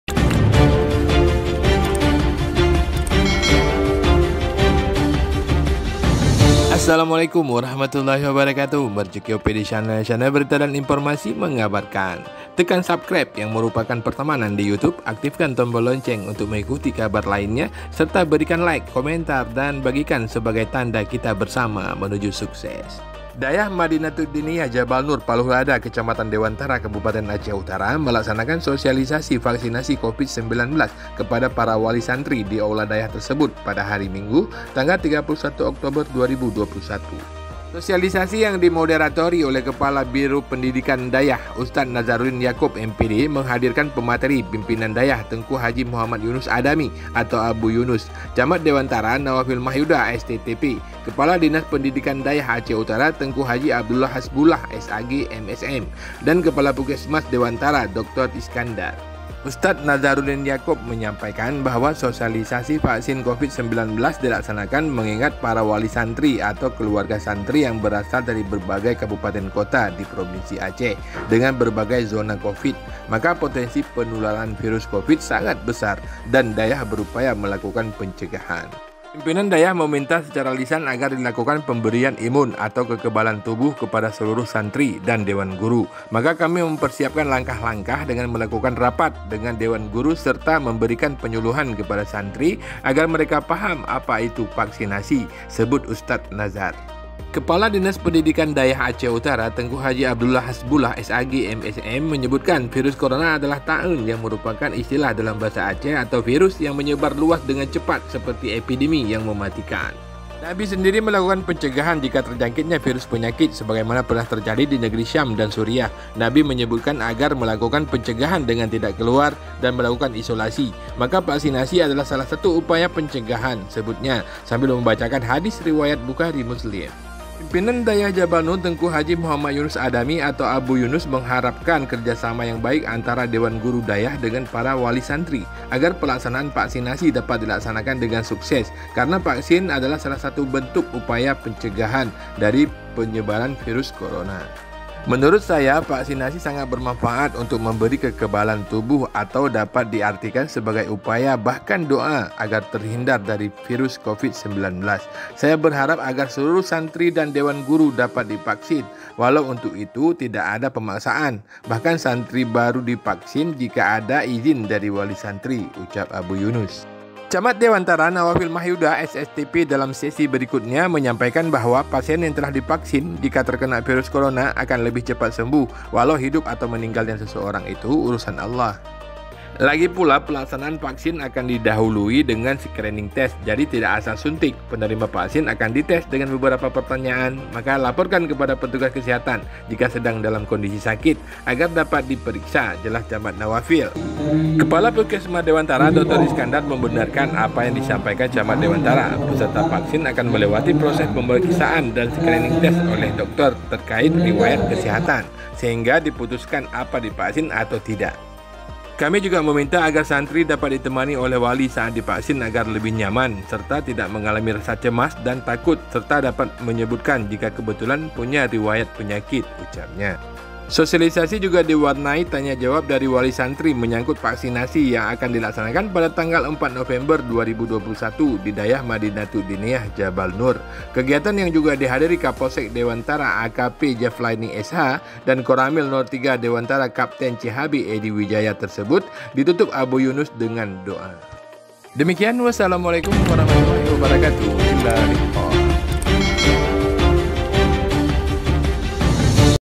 Sosialisasi Vaksin, Kadis Dayah Sebut Covid "TA'ÈT"
Sosialisasi Vaksin di Dayah, Kepala Dinas Sebut, Virus Corona “Ta’en”

Assalamua’alaikum Warahmatullahi wabarakatu, Marzuki OPD Channel, Channel Berita dan Informasi mengabarkan

Dayah Madinatuddiniah Jabal Nur, Paloh Lada, Kecamatan Dewantara, Kabupaten Aceh Utara, melaksanakan Sosialisasi Vaksinasi Covid-19 kepada para wali santri di aula dayah tersebut pada hari Minggu, 31 Oktober 2021.

Sosialisasi yang di mederatori oleh Kepala Biro Pendidikan Dayah Ustadz Nazaruddin Yacob, M.Pd, mengadirkan pemateri, Pimpinan Dayah, Tgk HM. Yunus Adami (Abu Yunus), Camat Dewantara, Nawafil Mahyudha, S.STP, Kepala Dinas Pendidikan Dayah Aceh Utara, Tgk H.Abdullah Hasbullah, S.Ag, MSM dan Kepala Puskesmas Dewantara, dr,Iskandar,

Ustadz Nazaruddin Yacob menyampaikan bahwa sosialisasi vaksinasi Covid-19 dilaksanakan mengingat para wali santri atau keluarga santri yang berasal dari berbagai Kabupaten/Kota di Provinsi Aceh dengan berbagai zona Covid, maka potensi penularan virus covid-19 sangat besar dan  dayah berupaya melakukan pencegahan.

“Pimpinan dayah meminta secara lisan agar dilakukan pemberian imun atau kekebalan tubuh kepada seluruh santri dan dewan guru, maka kami mempersiapkan langkah – langkah dengan melakukan rapat dengan dewan guru serta memberikan penyuluhan kepada santri, agar mereka faham apa itu vaksinasi”, Sebut Ustadz Nazaruddin,
Kepala Dinas Pendidikan Dayah, Aceh Utara, Tgk.H.Abdullah Hasbullah, S.Ag.MSM menyebutkankan, Virus Corona adalah “Ta”en” yang merupakan istilah dalam bahasa Aceh atau virus yang menyebar luas dengan cepat seperti epidemi yang mematikan.
“Nabi sendiri melakukan pencegahan jika terjangkitnya virus penyakit, sebagaimana pernah terjadi di negeri Syam dan Suriah, nabi menyebutkan agar melakukan pencegahan dengan tidak keluar dan melakkan isolasi, maka vaksinasi adalah salah satu upaya pencegahan, Sebutnya, sambil membacakan hadist riwayat bukhari muslim
Pimpinan Dayah Jabal Nur, Tgk H.M. Yunus Adami (Abu Yunus), mengaharapkan kerja sama yang baik antara dewan guru dayah dengan para wali santri, agar pelaksanaan vaksinasi dapat dilaksanakan dengan sukses, karena vaksin adalah salah satu bentuk upaya pencegahan dari penularan virus corona.

“Menurut saya, vaksinasi sangat bermanfaat untuk memberi kekebalan tubuh atau dapar diartikan sebagai upaya bahkan do’a agar terhiindar dari virus covid-19, saya berharap agar seluruh santri dan dewan guru dapat di vaksin walau untuk itu tidak ada pemaksaan, bahkan santri baru divaksin jika ada izin wali santri” ucap Abu Yunus.
Camat Dewantara, Nawafil Mahyudha, S.STP, dalam sesi berikutnya menyampaikan bahwa pasien yang telah divaksin, jika terkena virus corona akan lebih  cepat sembuh, walau hidup atau meninggalnya seseorang itu urusan Allah.

“Lagi pula pelaksanan vaksin akan didahului dengan screening test, jadi tidak asal suntik, penerima vaksin akan ditest dengan beberapa pertanyaan, maka laporkan kepada petugas kesehatan jika sedang dalam kondisi sakit agar dapat dapat diperiksa”, jelas, Camat Nawafil.
Kepala Puskesmas Dewantara, dr Iskandar membenarkan apa yang disampaikan Camat Dewantara, peserta vaksin akan melewati proses pemeriksaan dan sreening oleh dokter terkait riwayat kesehatan, sehingga diputuskan apa divaksin atau tidak..

“Kami juga meminta agar santri dapat ditemani oleh wali saat divaksin agar lebih nyaman, serta tidak mengalami rasa cemas dan takut serta dapat menyebutkan jika kebetulan punya riwayat penyakit, ucapnya.

Sosialisasi juga diwarnai tanya jawab dari wali santri menyangkut vaksinasi yang akan dilaksanakan pada tanggal 04 November 2021 di Dayah Madinatuddiniah Jabal Nur, Kegiatan yang juga dihadiri Kapolsek Dewantara AKP Zaflaini, SH dan Koramil 03 Dewantara, Kapten Chb. Edi Wijaya tersebut ditutup Abu Yunus dengan do’a.

Demikian, Wasalamualaikum warahmatullahi wabarakatu, ilalliqa